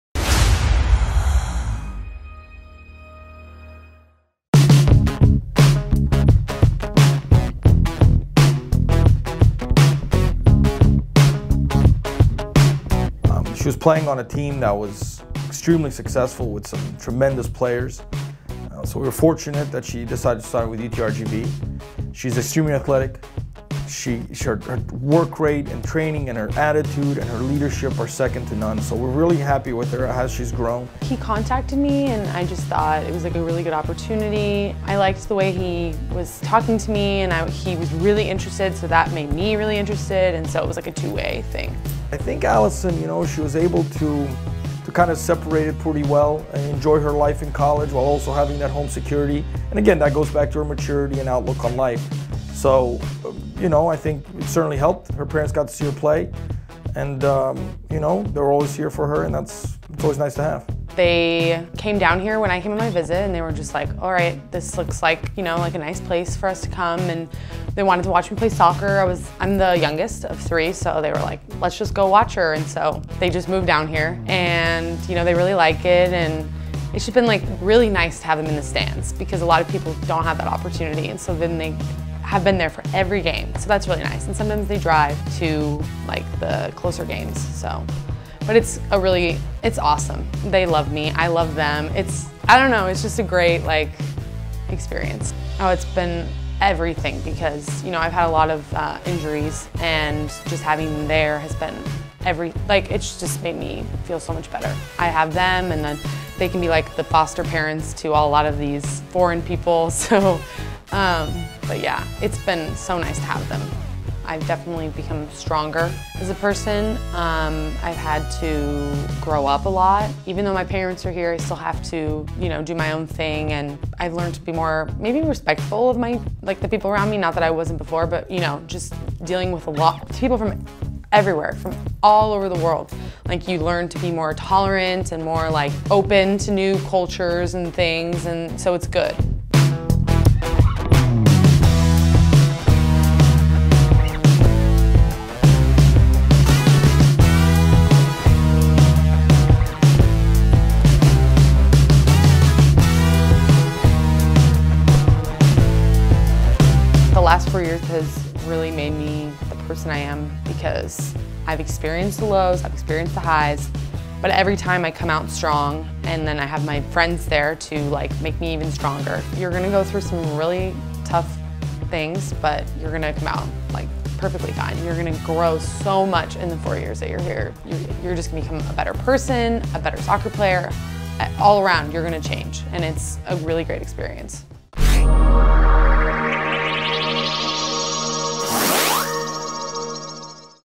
The Smiths: From SoCal to the RGV
Oct. 9, 2017 – EDINBURG, Texas – Soccer has been a staple in the life of UTRGV senior women’s soccer forward Allyson Smith and her family.

Smith’s brother played a lot of soccer growing up and that caused her passion for the game to grow.

“My older brother played soccer so I would always go to his games,” Smith said. “I was always carried around there and I figured I might as well play this.”

Smith started playing when she was around three years old and she always looked up to her brother, who set the right example for her on and off the field. Some of her fondest memories of playing soccer are those times when her brother would show up to her games.

Family is important for Smith and she is important to her family. Proof of that was when Smith decided to come to UTRGV. After her four years at Mater Dei High School, Smith had to make a decision on where she wanted to move to after calling Orange, California home for so long.

Smith did her research on the Rio Grande Valley (RGV) and saw that the region was growing and would be a nice place to live. There was mutual interest from UTRGV head coach Glad Bugariu.

Smith came to the RGV for a visit, but she didn’t come alone as her parents also made the trip. Smith wasn’t the only one deciding whether or not to move to the RGV.

“They came down here when I came on my visit,” Smith said. “They were like alright, it looks like a nice place for us to come. They wanted to watch me play soccer. I am the youngest of three and they were like, ‘let’s just go watch her,’ so they just moved down here.”

Smith is grateful to be able to have her family in the stands for every home match. She knows not everyone gets that opportunity. Not only have her parents been there to show support in the stands, but they have been there to show support through all the tough times as well.

Smith’s parents have been such a benefit for Smith, but she also says that her parents have taken in some of her teammates as well.

For four years now, Smith and her parents have adjusted well to the RGV. The biggest differences for them have been the weather and the food, but Smith says the people are nice and welcoming in the RGV, just like back at home.

“It’s cool to live in a different place,” Smith said. “Everywhere you go there is a little bubble in the community, and this is just a different bubble of the United States. It’s been cool to have that experience.”

While she has so many things to be thankful for since moving down to the RGV, Smith is most thankful for her friends. She has been able to learn more about different cultures with so many teammates coming from different parts of the world, allowing Smith to grow as a person.

Four years ago, she came in a freshman, but Smith wasn’t the only one. She got to meet two other freshmen with whom she formed a bond with that will last a lifetime. Smith holds all her teammates in high regards, but her relationship with fellow seniors Hanna Spets and Frida Farstad-Eriksson is beyond special.

“We have been through so much together,” Smith said. “We are among the six remaining who started the program together. Those two are home for me. Everywhere we go we are always together. They are my best friends.”

The three of them, along with Marcela Ramirez, Erica Gonzalez and Andreya Barrera, are the remaining players who started this program. Each one of the six has experienced all the ups and downs of the women’s soccer program, but Smith still says they are still a pretty strong group.

Looking back to her four years at UTRGV, Smith has grown in every facet of her life, becoming a better person, teammate, daughter and friend. A big reason for her growth has been the leadership that she has experienced from day one of head coach Glad Bugariu and assistant coaches Lindsay Vera and Silviu Telespan.

“They are always there for us,” Smith said. “They have seen everything we have gone through and the people we have become because we have all grown so much since freshman year. I can’t even imagine being a freshman again because I don’t know how I dealt with it or how the six of us dealt with it. The coaches have been there every step of the way with us.”

Smith is majoring in pre-med biology and she is still considering her options for her next step after graduation. She is thinking about medical school and PA school.

Smith is glad she chose to move to the RGV and attend UTRGV. The last four years has shaped her life and she wouldn’t change a thing.

“The last four years have made me the person that I am right now,” Smith said. “I have experienced the lows and I have experience the highs, but every time I come out strong. Then I have my friends there to make me even stronger.”